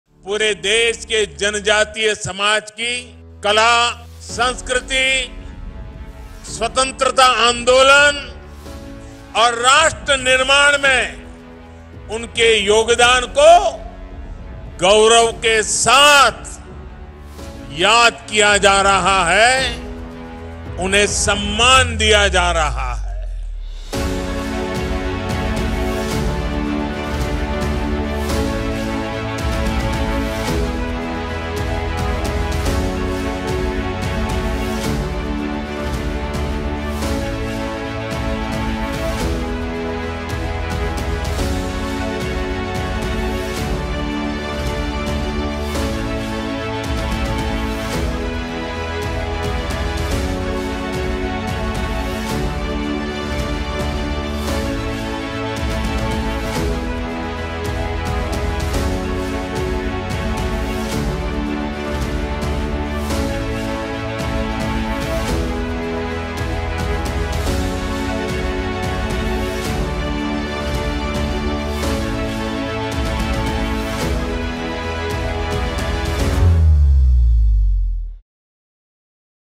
जनजातीय समुदायों को दशकों तक नहीं मिला सम्मान, मोदी सरकार ने लौटाया उनका खोया अभिमान
जनजातीय समुदायों को दशकों तक नहीं मिला सम्मान,

मोदी सरकार ने लौटाया उनका खोया अभिमान।